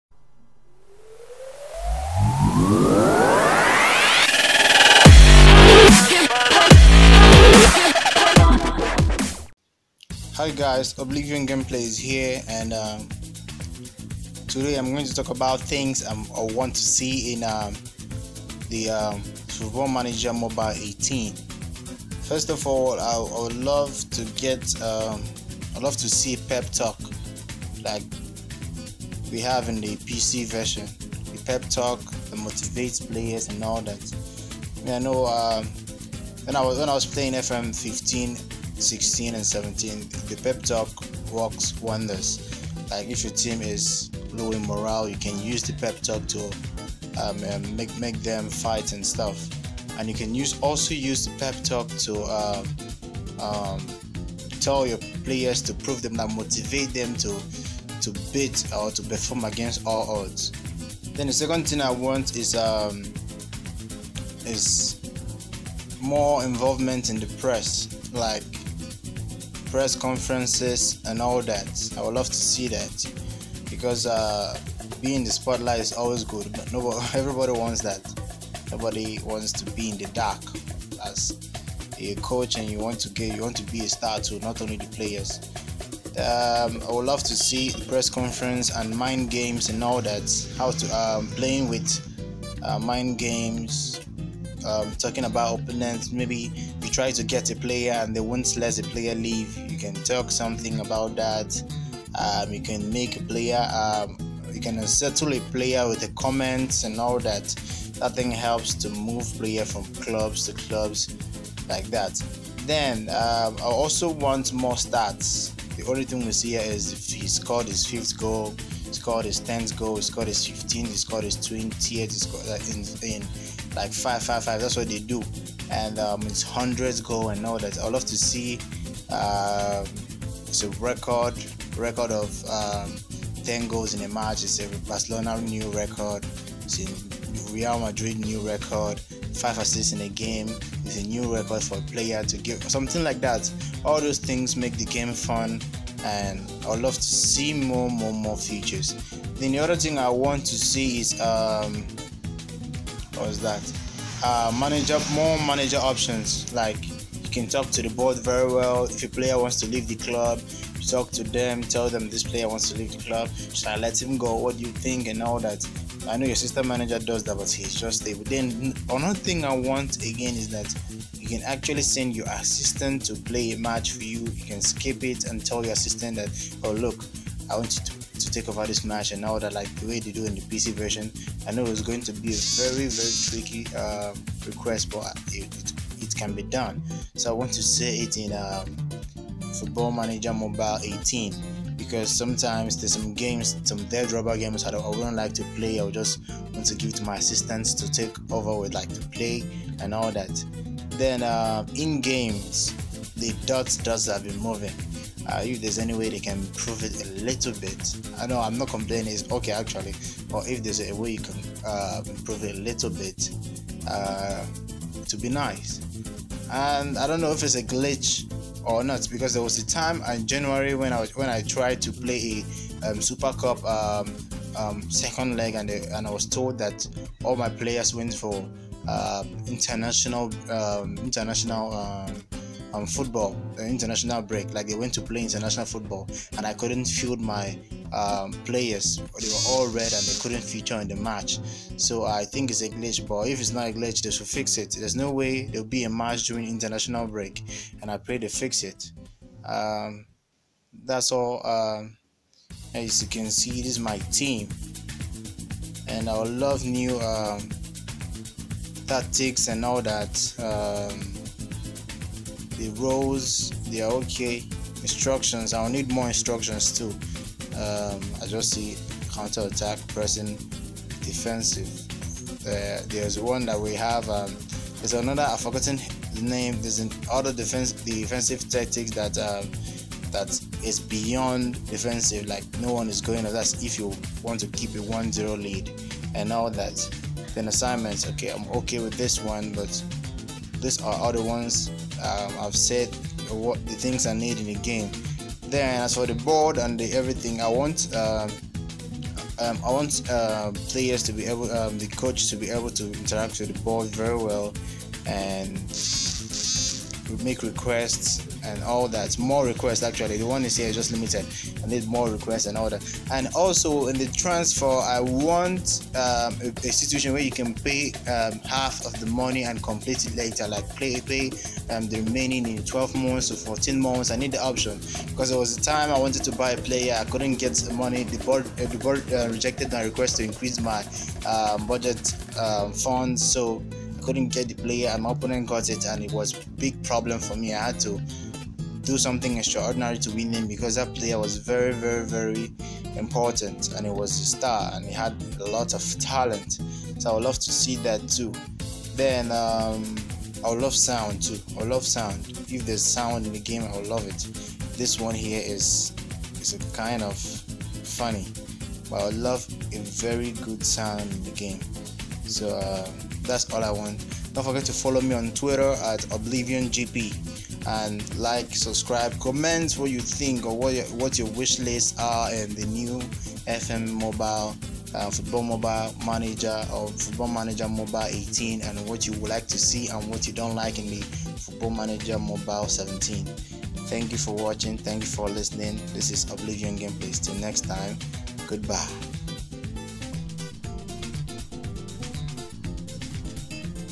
Football Manager Mobile 2018 FMM18| WISHLIST AND OTHERS
As we are waiting for the Football Manager Mobile 2018, I made a video to talk about things i would love to see in the upcoming version and areas that needs to be worked on.